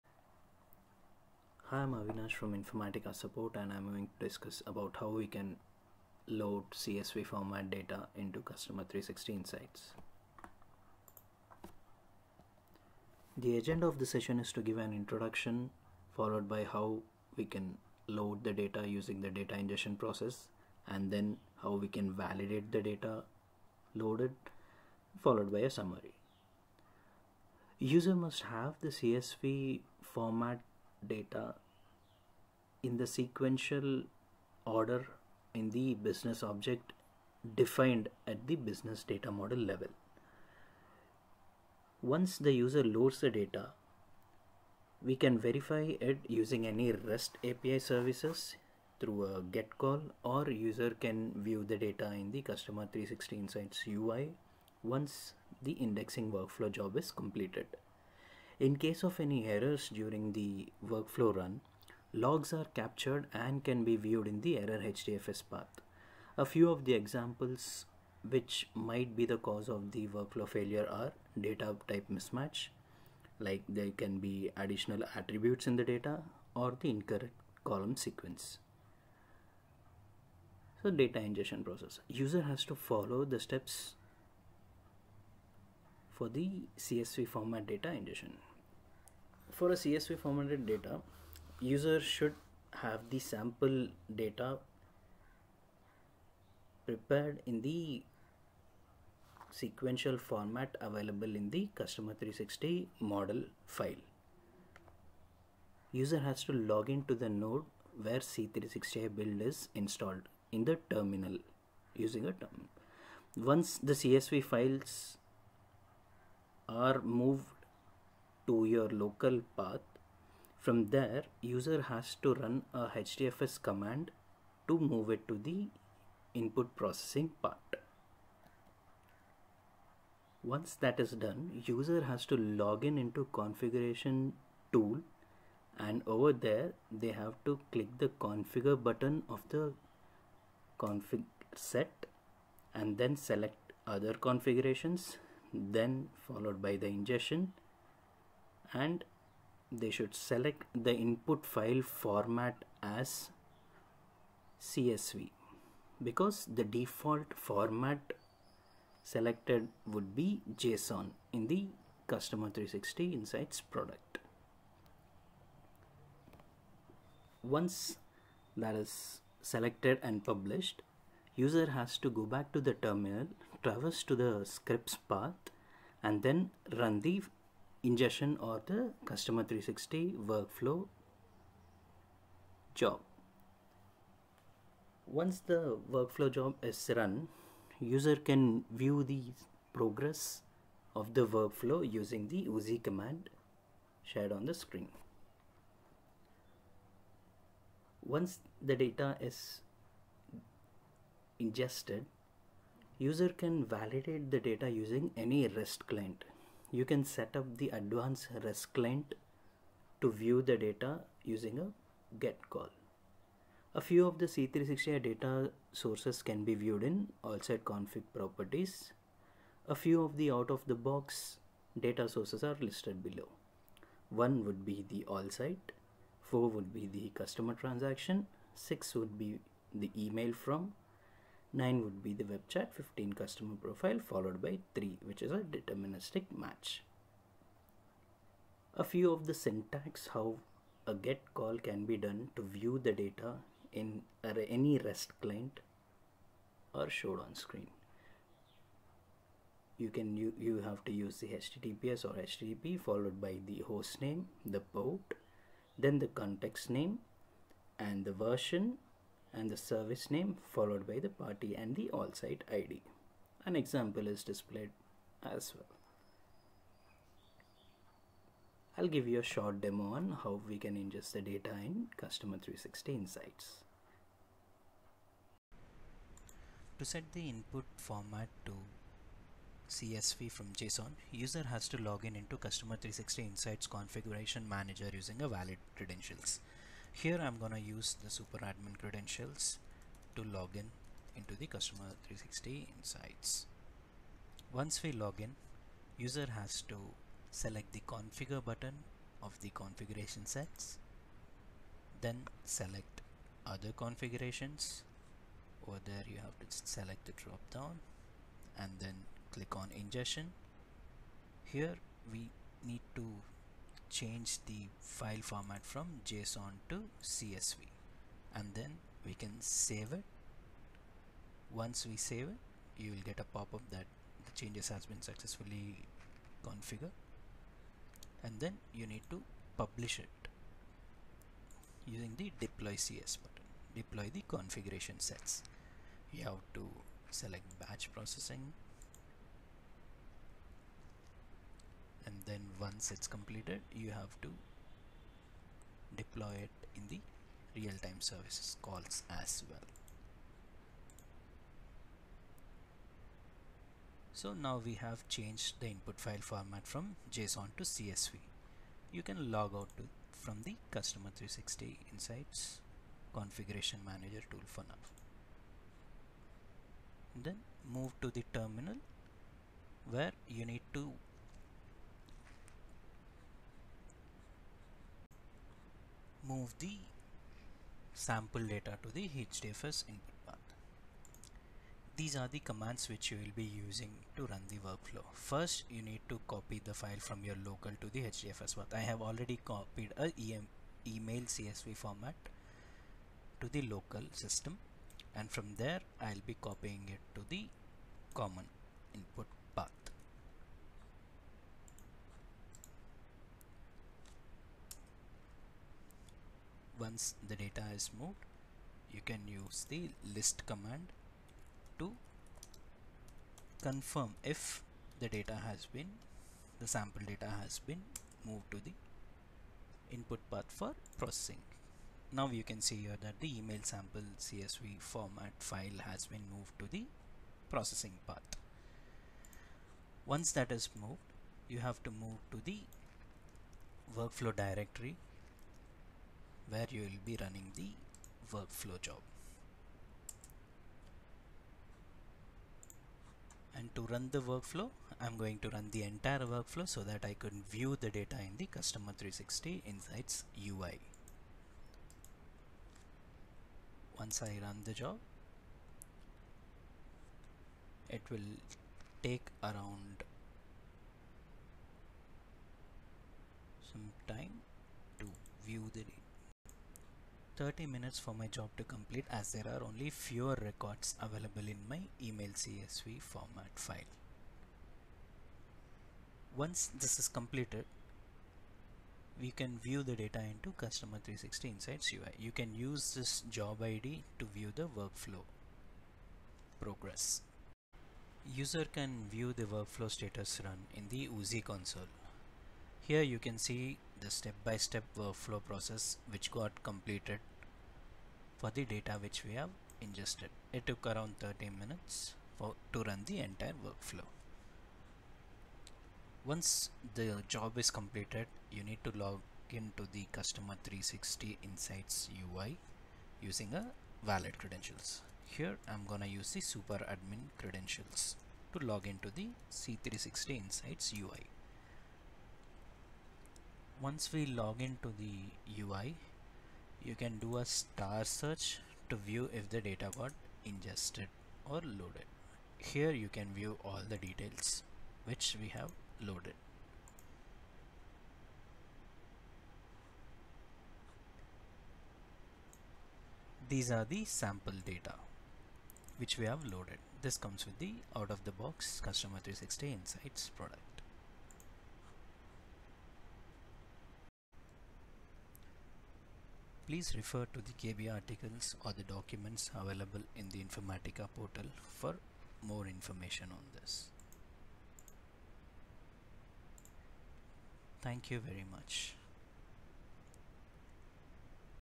How to Load CSV Format Data Ingestion into Customer 360 Insights
This video explains how to load CSV Format Data Ingestion into Customer 360 Insights.

Expertise: Beginner
User type: Administrator, Data Steward, Data Analyst, Developer
Category: Configuration
Project Phase: Configure
Problem Type: Configuration